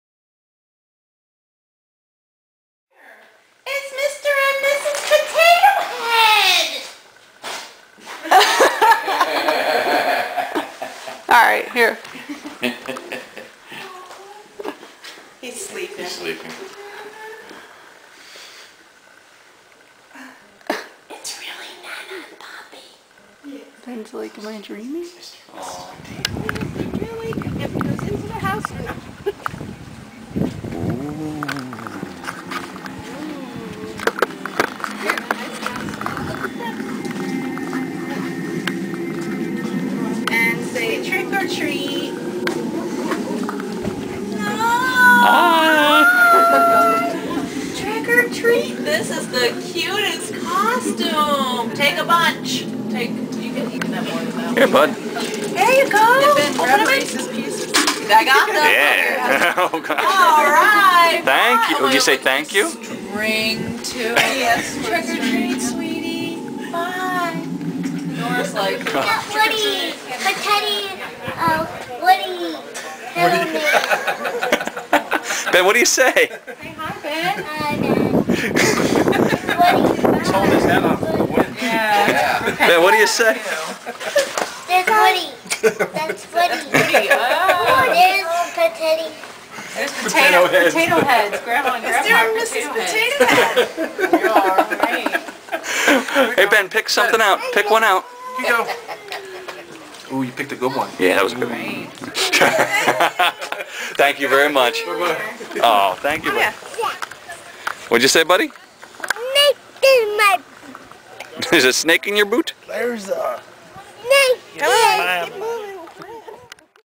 2009-10-31-Halloween Bonus footage
Here's some extra video I found of Ben trick-or-treating with the family! My mom and step-father both dressed up as Mrs. and Mr. Potato Head from Toy Story. Ben loved it!